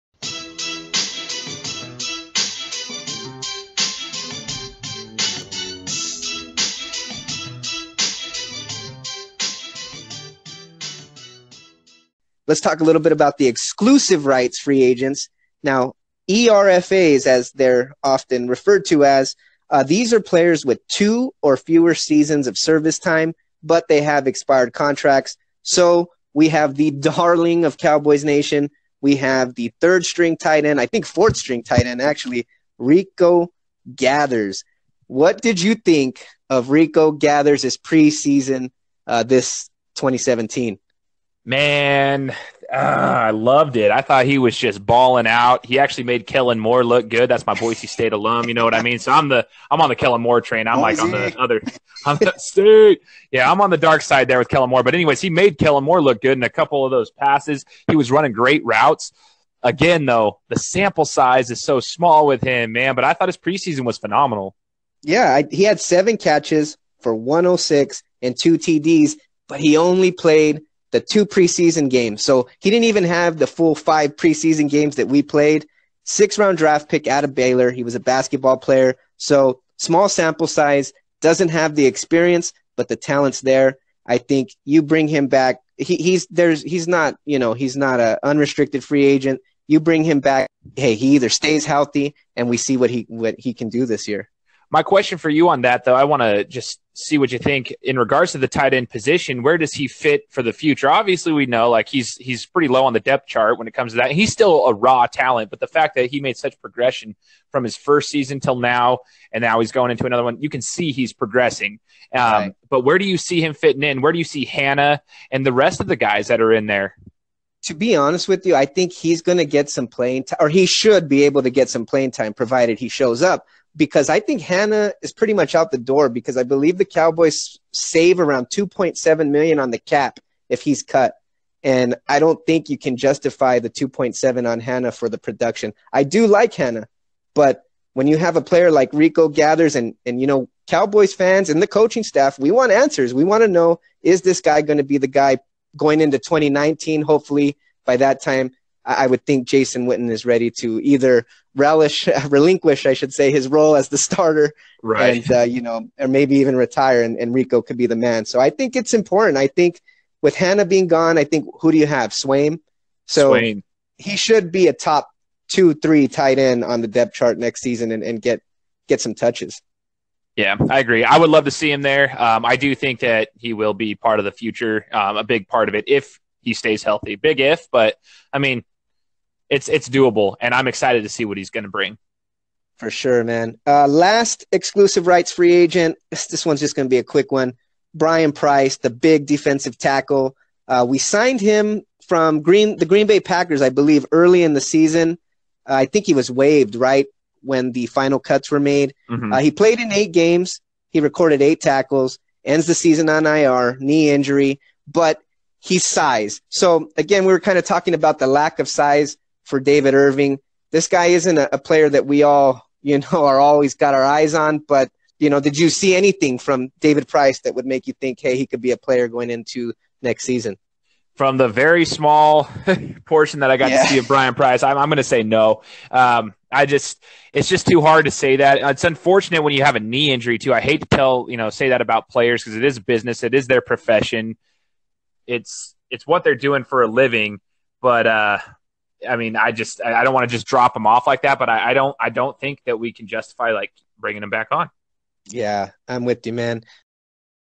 EP2: Exclusive Rights Free Agents - Rico Gathers (TE), Brian Price (DT)
The SayWhatchaLike Sports Podcast presents...The 2018 Dallas Cowboys Free Agency Breakdown. A 12 episode series that breaks down every player on the Dallas Cowboys Roster going into the off season without a contract. In this episode Phillip Enriquez (SayWhatchaPhil) & Shane Larson (The Gametime Guru) break down the players (Rico Gathers & Brian Price) who are the EXCLUSIVE RIGHTS Free Agents for the Dallas Cowboys this offseason. Thanks again!